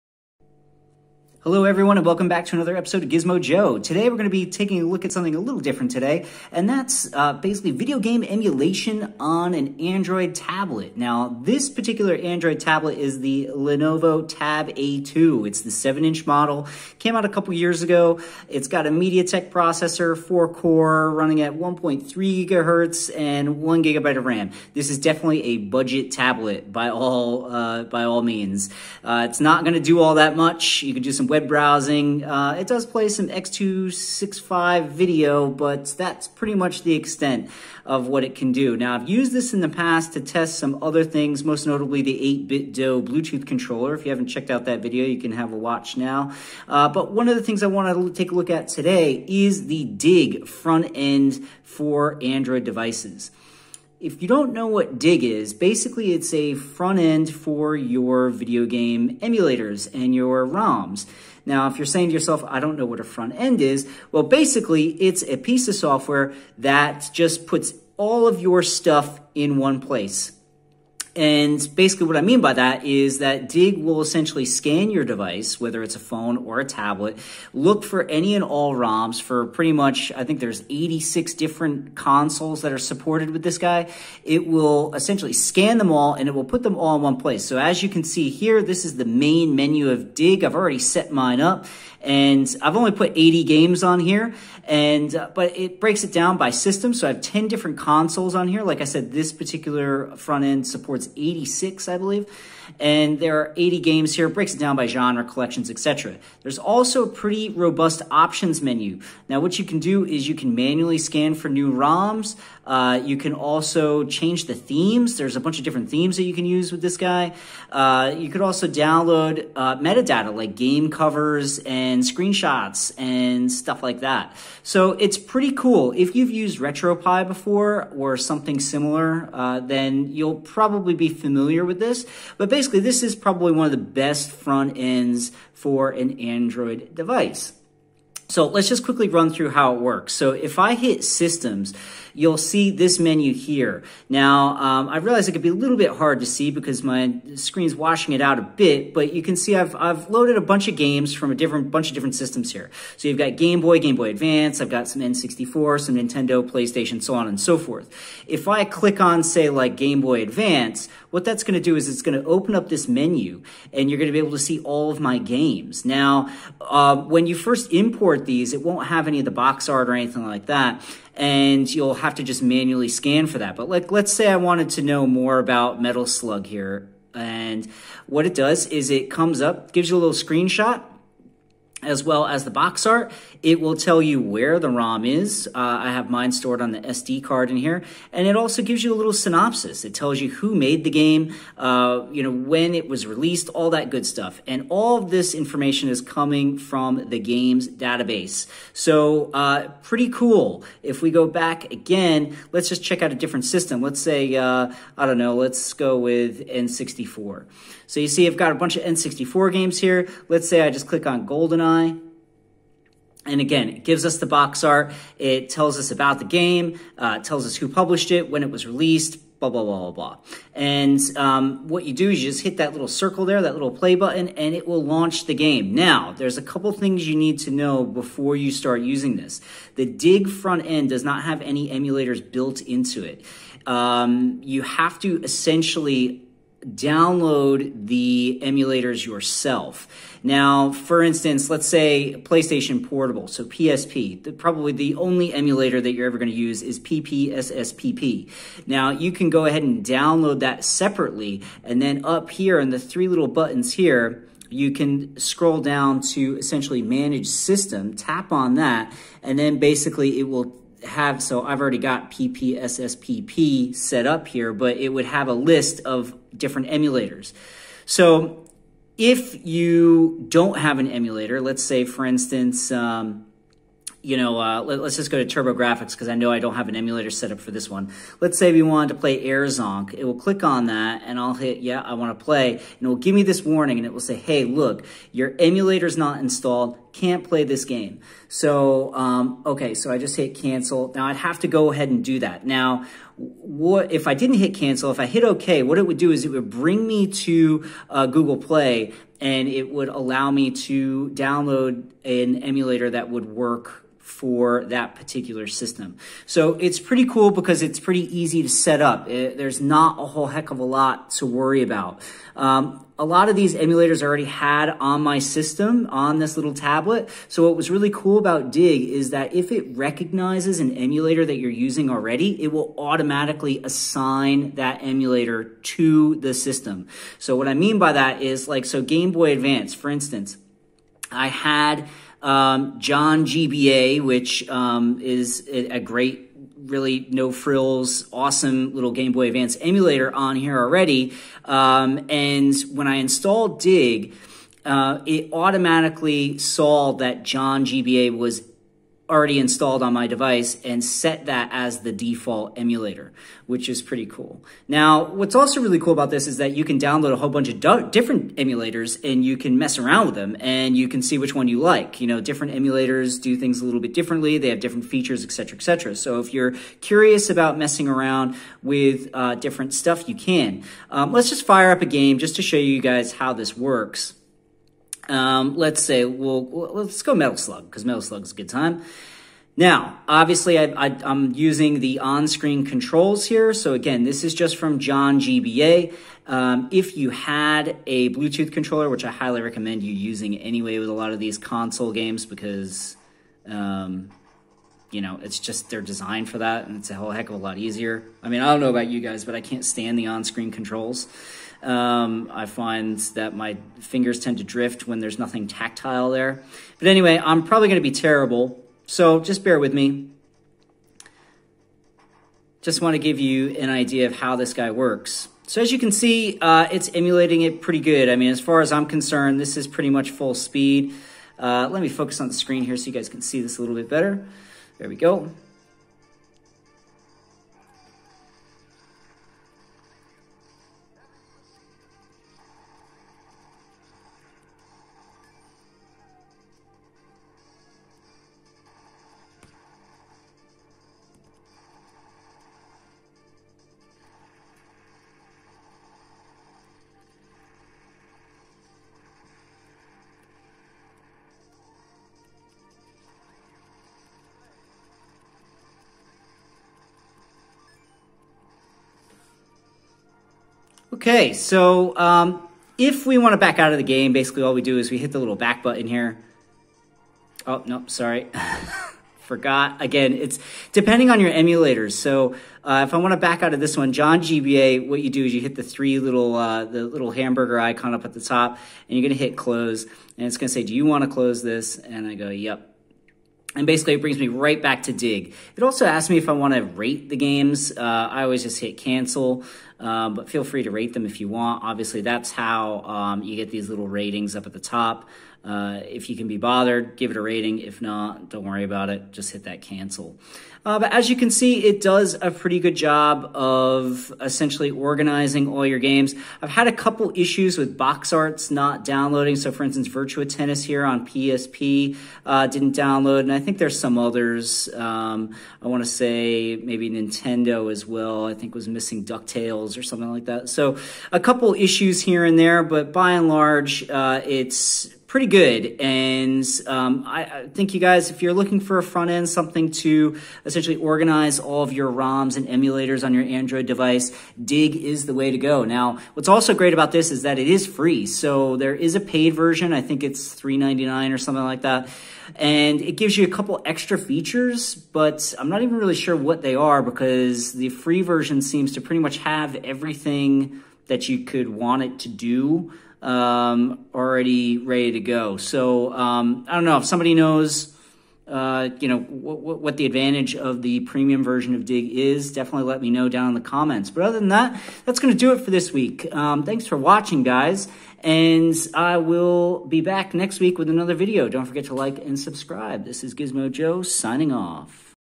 DIG Frontend for Android Emulation
DIG is a fantastic emulator front-end for Android devices. In this video, I'll break down some of its features.